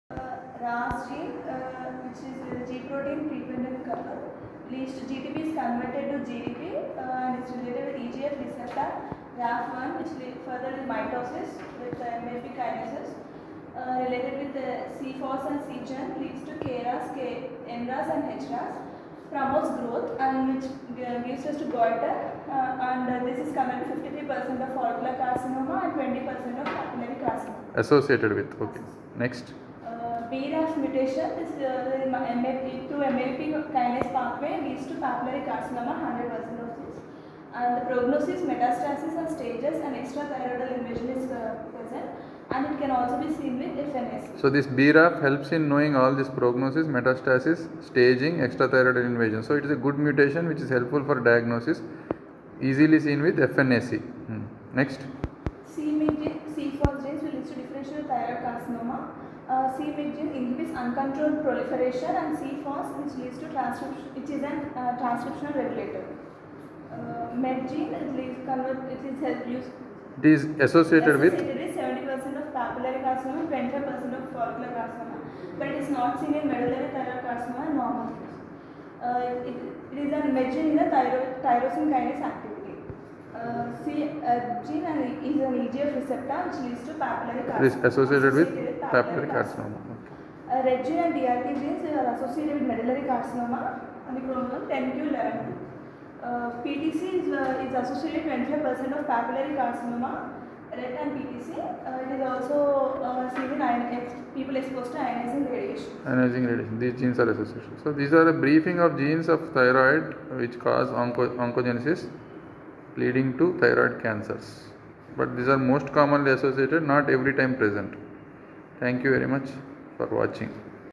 THYROID TUMORS -ONCOGENESIS
THYROID ONCOGENESIS HAS FEW GENES TO BE KNOWN ,WHETHER IT BECOMES PAPILLARY OR FOLLICULAR OR ANAPLASTIC .THIS VIDEO TEACHES US THE SAME